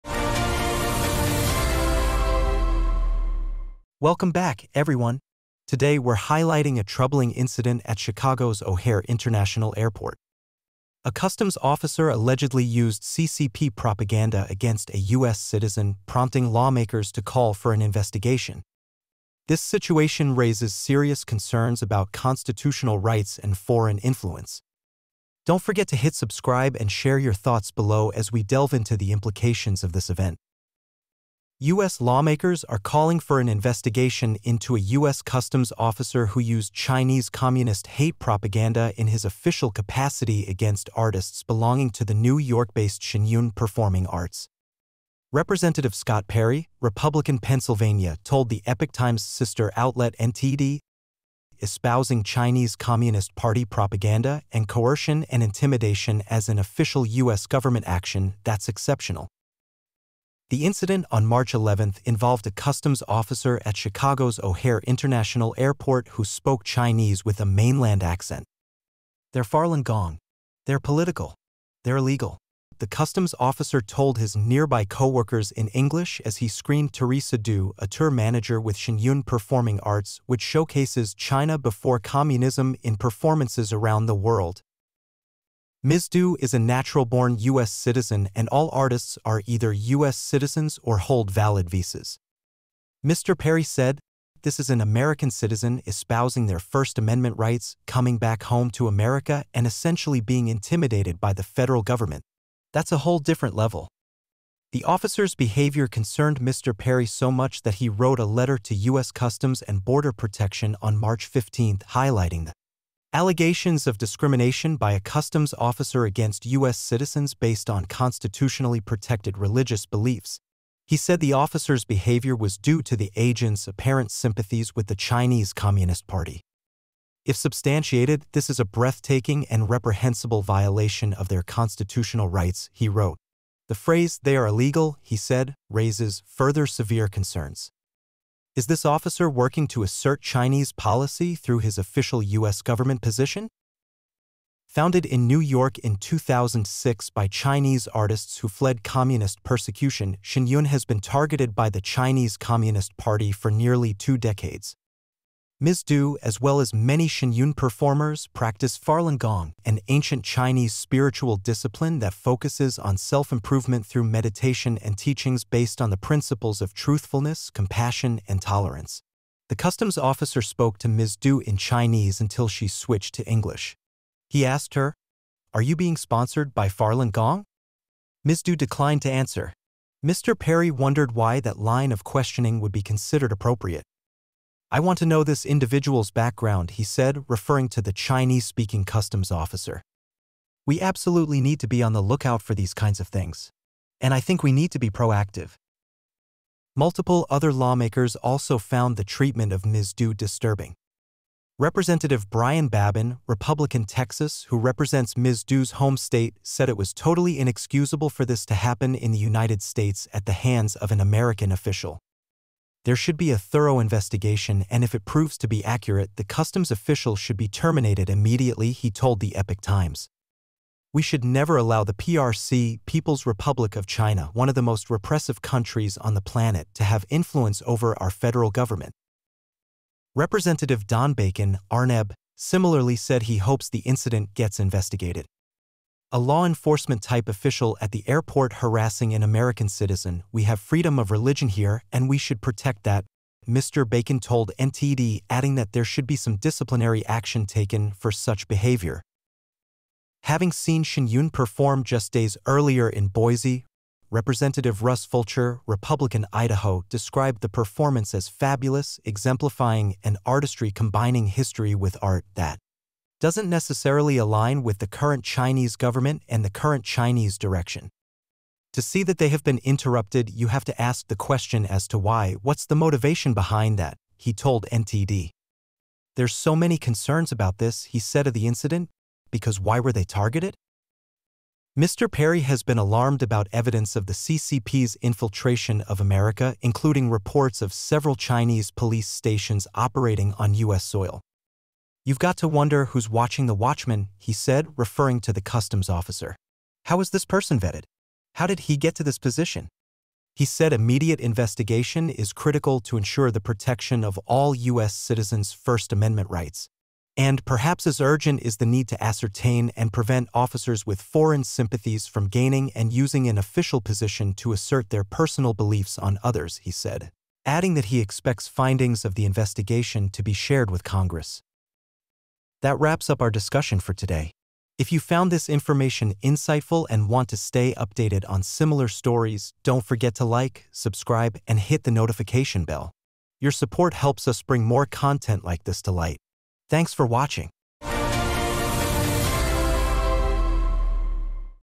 U.S. Customs Officer's Shocking Use of CCP Propaganda Against American Citizen ｜China Pulse
Today, we're highlighting a troubling incident at Chicago's O’Hare International Airport. A customs officer allegedly used CCP propaganda against a U.S. citizen, prompting lawmakers to call for an investigation. This situation raises serious concerns about constitutional rights and foreign influence.
CCP Propaganda, U.S. Customs Officer, Constitutional Rights